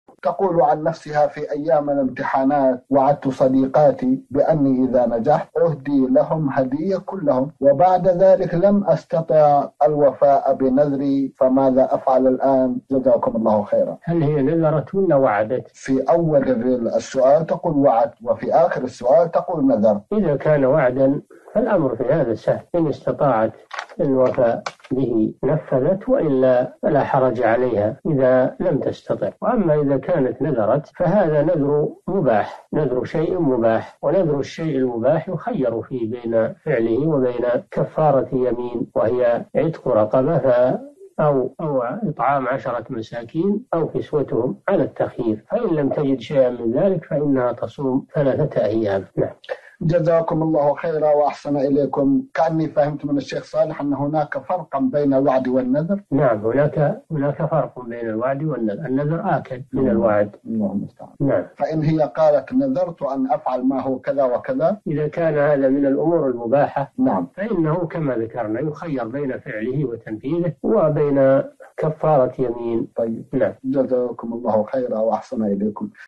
وعدت صديقاتي بأني إذا نجحت أهدي لهم هدية كلهم، وبعد ذلك لم أستطع الوفاء بنذري، فماذا أفعل الآن؟
نص السؤال: في أيام الامتحانات وعدتُ صديقاتي بأني إذا نجحت أُهدي لهم هدية كلهم، وبعد ذلك لم أستطع الوفاء بنذري، فماذا أفعل الآن؟
السيرة الذاتية :
للاشتراك في قناة الشيخ صالح الفوزان

رئيس الفريق
لمشروع كبار العلماء